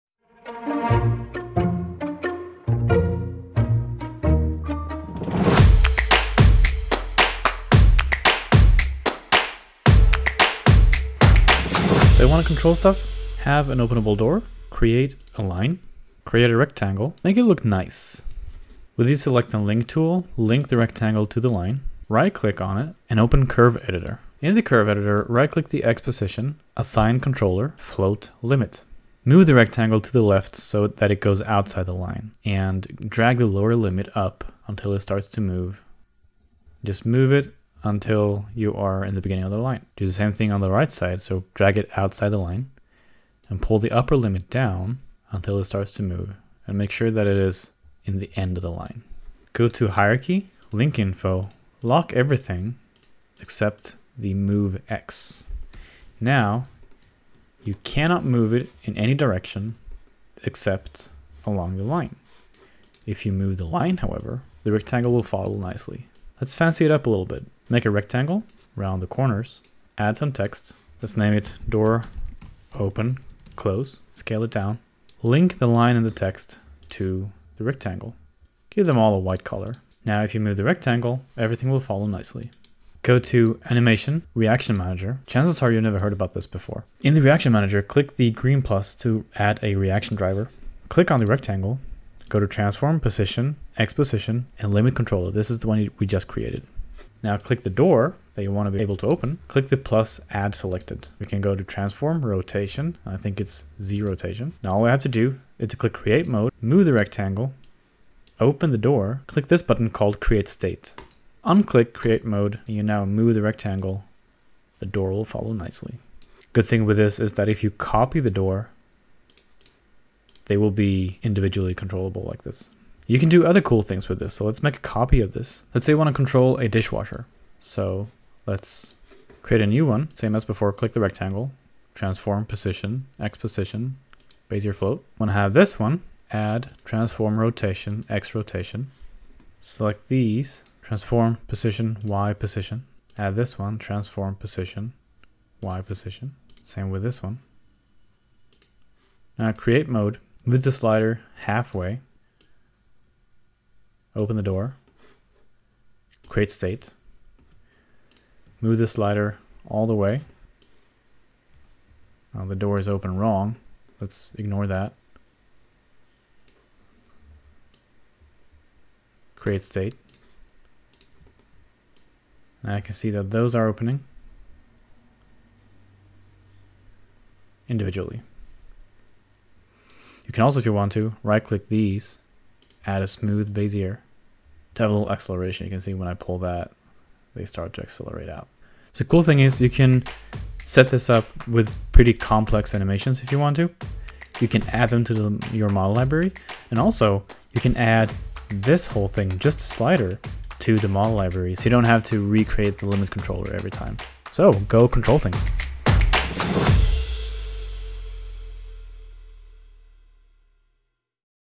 Slider controllers using "Reaction Manager" in 3Ds Max
So I've wanted to figure this shit out for a long time. Never really did however, and the built in sliders in 3Ds Max are just worthless.

Enough was enough and I set out to finally crack this nut and as it turns out, it's really damn easy. To save you some time, here's the most straight forward and condensed tutorial I've ever done. Took me way beyond an hour to figure out, and I'm teaching it to you in 3,5 minutes.

Cheers, people, you're all wonderful!